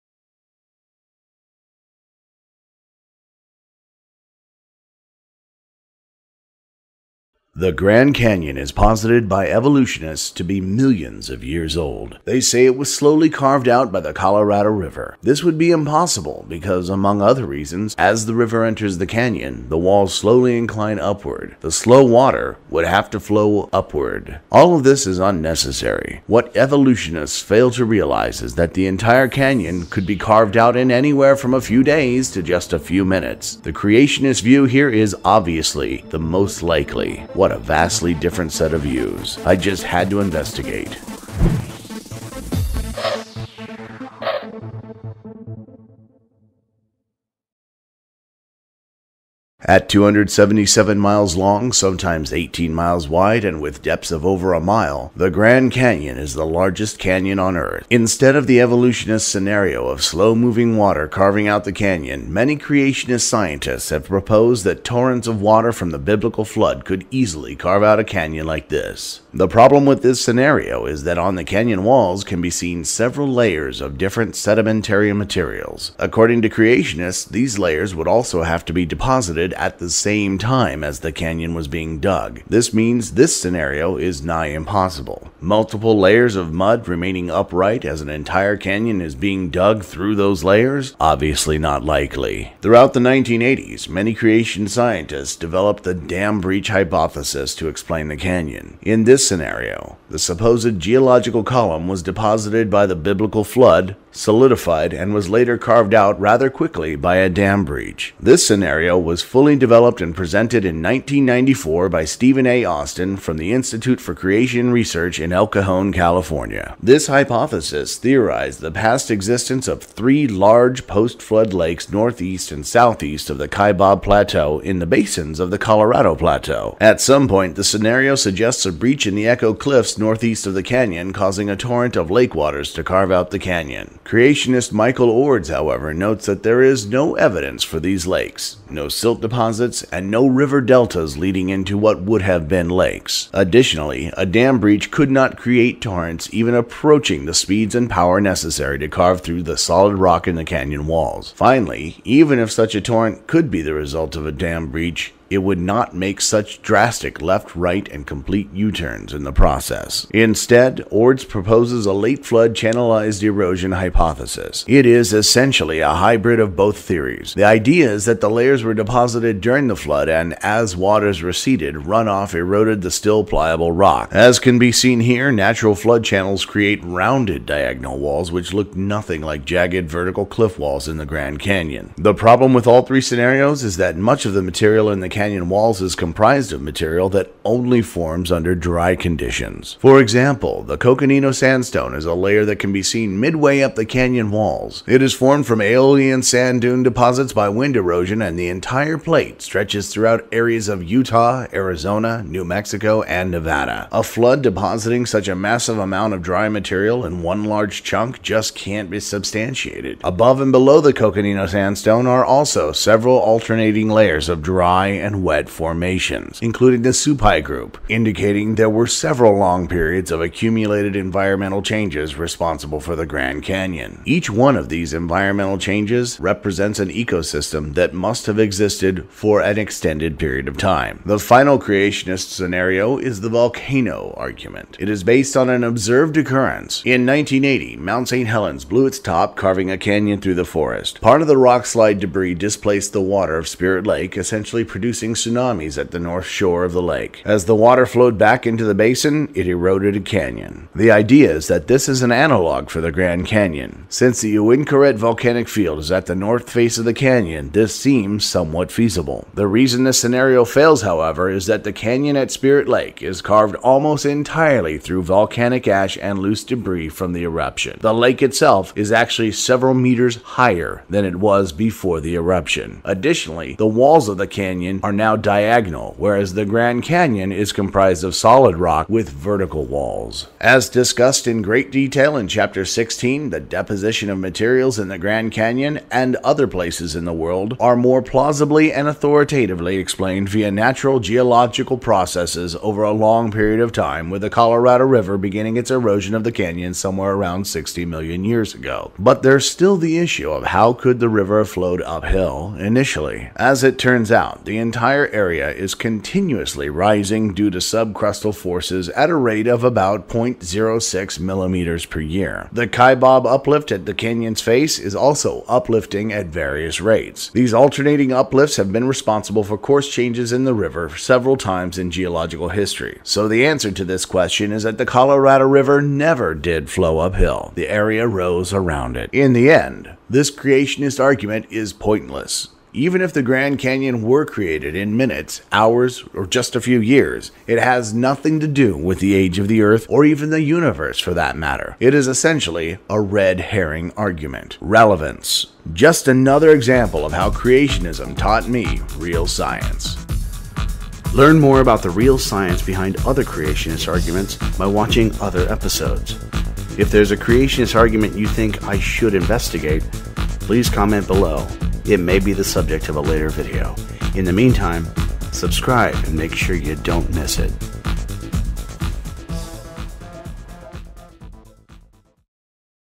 How Creationism Taught Me Real Science 23 The Grand Canyon
In this episode we examine the Grand Canyon and see if it really took millions of years to form.

Michael Oards' refutation of the Dam-Breach hypothesis and evidence for late Flood channelized erosion.

Details of the Canyon
Kiver, E.P.; Harris, D.V. (1999). "Geology of US Parklands"

Supai Group
Blakey, and Ranney, (2008). "Ancient Landscapes of the Colorado Plateau"